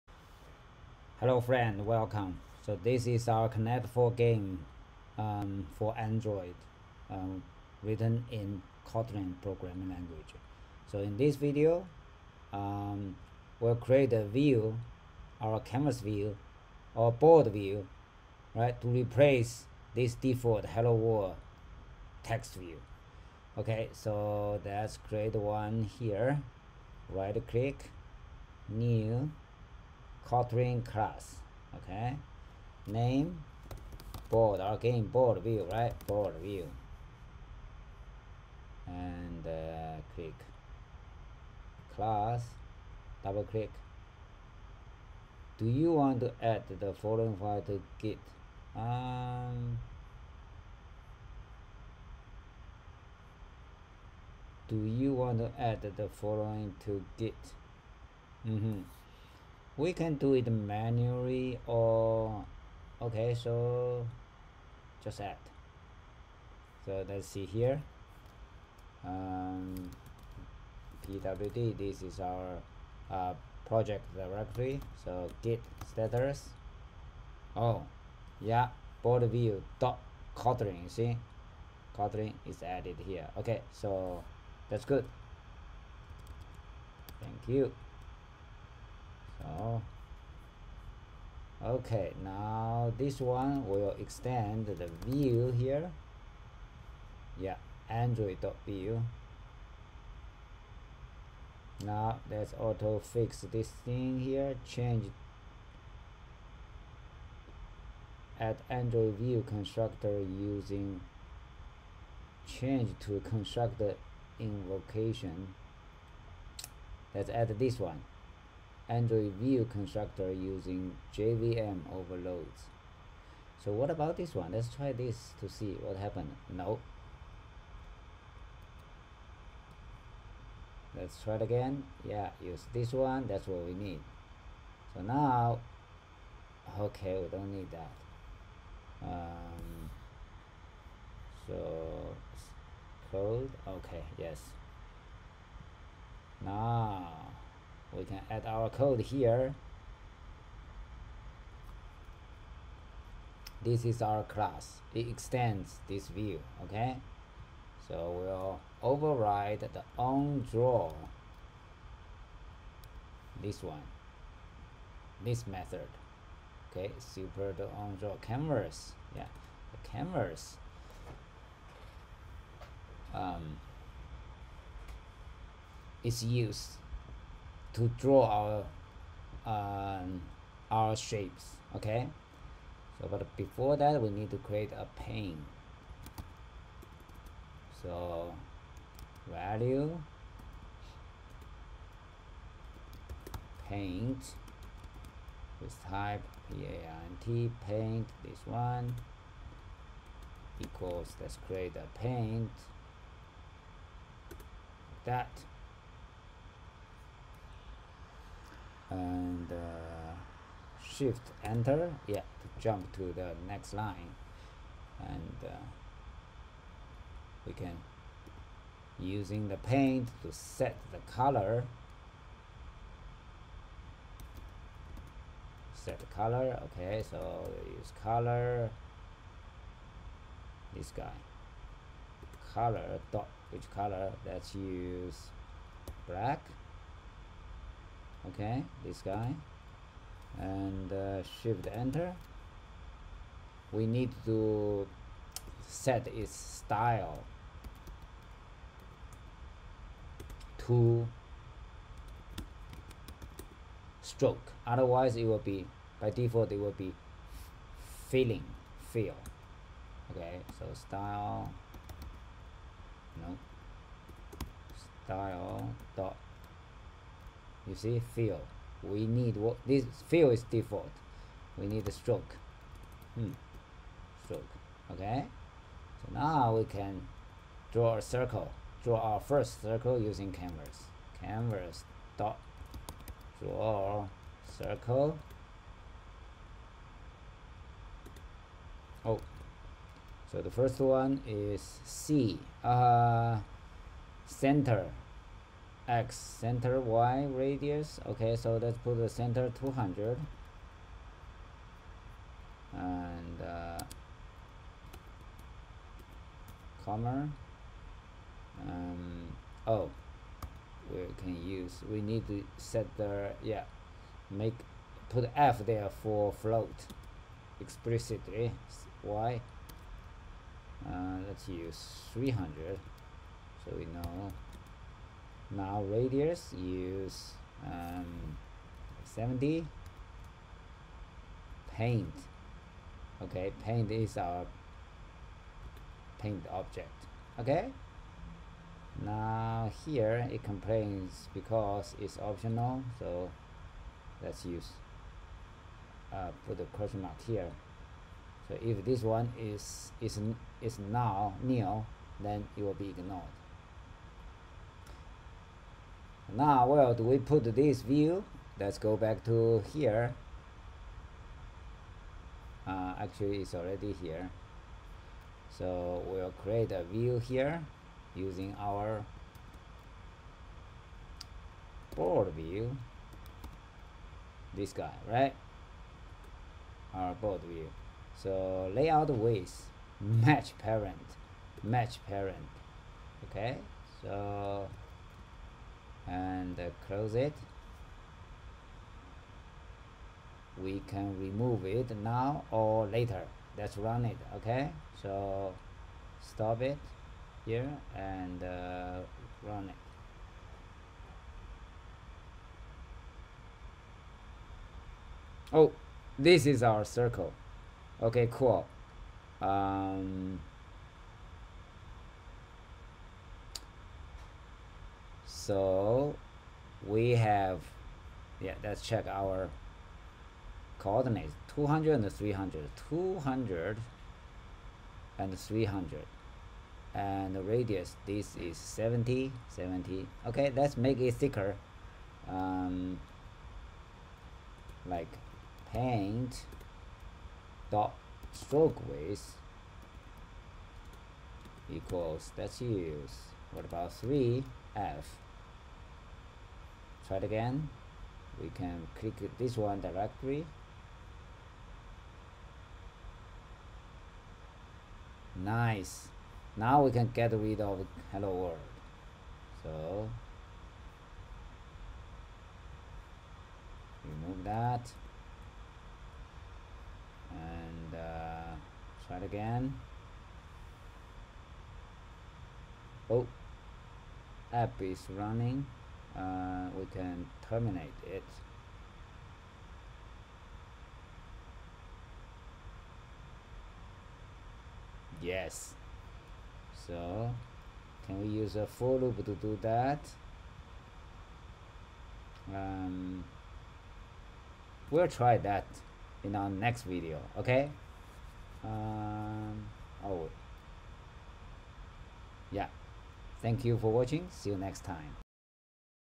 Android Conn4 002: Drawing a circle on BoardView in Kotlin
Kotlin code to draw a circle:
        val paint: Paint = Paint()
        paint.setColor(Color.BLACK)
        paint.strokeWidth = 3f
        paint.style = Paint.Style.STROKE
        canvas?.drawCircle(200f, 300f, 70f, paint)